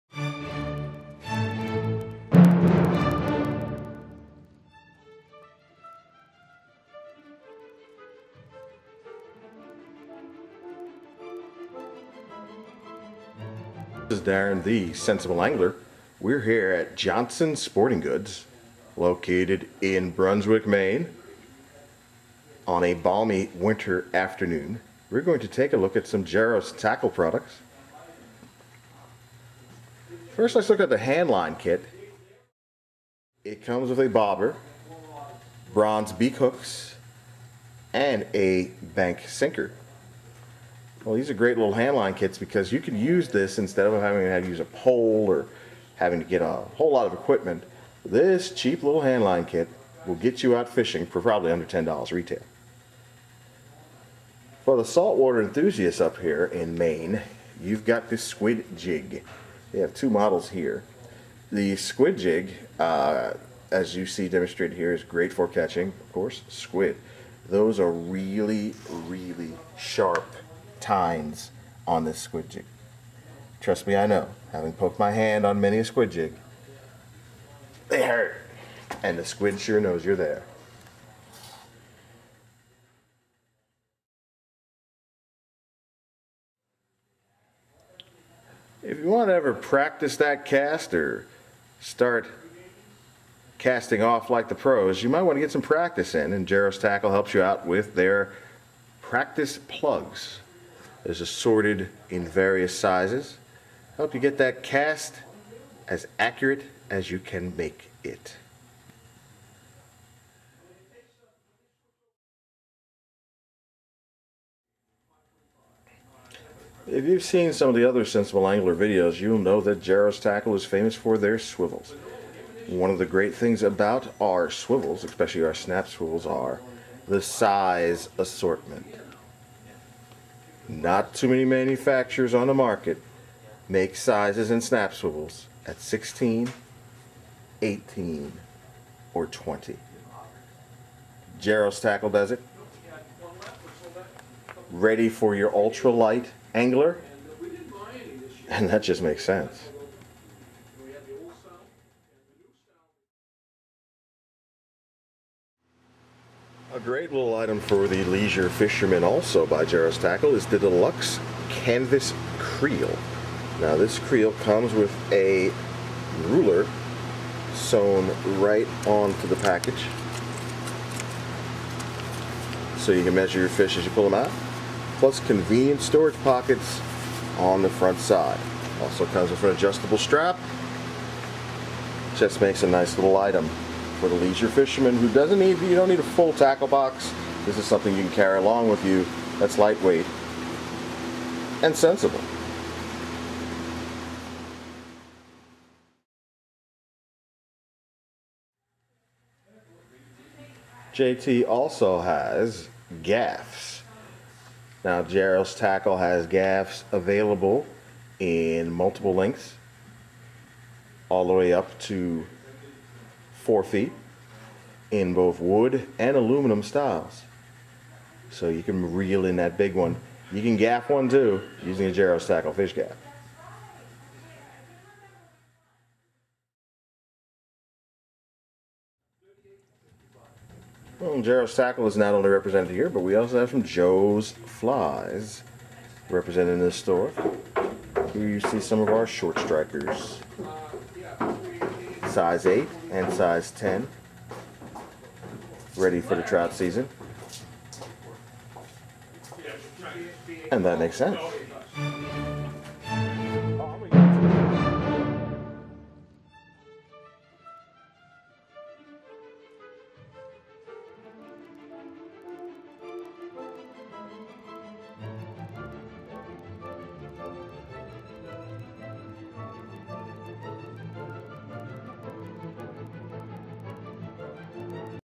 JOHNSON SPORTING GOODS PART 1
The Sensible Angler goes to Brunswick, Maine and reviews products at Johnson's Sporting Goods; a family-owned and run three store sporting goods chain.